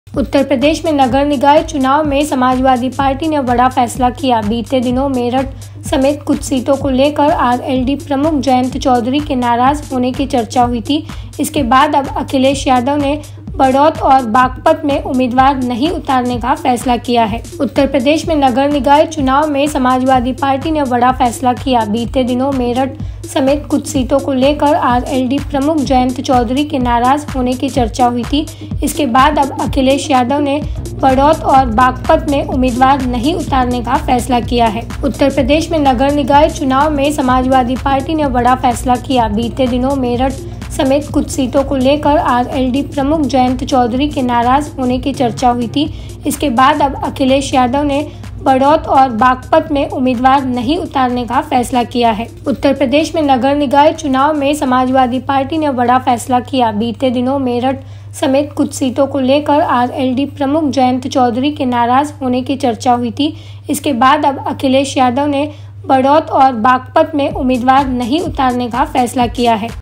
Akhilesh Yadav ने बडौत और बागपत में उम्मीदवार नहीं उतारने का फैसला किया
उत्तर प्रदेश में नगर निकाय चुनाव में समाजवादी पार्टी (Samajwadi Party) ने बड़ा फैसला किया है. बीते दिनों मेरठ (Meerut) समेत कुछ सीटों को लेकर आरएलडी (RLD) प्रमुख जयंत चौधरी (Jayant Chaudhary) के नाराज होने की चर्चा हुई थी. इसके बाद अब अखिलेश यादव (Akhilesh Yadav) ने बडौत (Baraut) और बागपत (Baghpat) में उम्मीदवार नहीं उतारने का फैसला किया है.

सपा ने गठबंधन दलों की नाराजगी के बीच कुछ सीटों पर उम्मीदवार नहीं उतारने का फैसला किया है. पार्टी ने पश्चिमी यूपी के बड़ौत और बागपत नगर निकाय चुनाव में अध्यक्ष पद के लिए उम्मीदवार नहीं उतारने का फैसला किया है. इसकी जानकारी देते हुए पार्टी ने ट्वीट कर लिखा, "बड़ौत और बागपत में समाजवादी पार्टी स्थानीय नगर निकाय चुनाव में अध्यक्ष पद हेतु चुनाव नहीं लड़ेगी और राष्ट्रीय लोकदल का समर्थन करेगी." यानी अब बागपत और बड़ौत की सीट पर आरएलडी का उम्मीदवार होगा.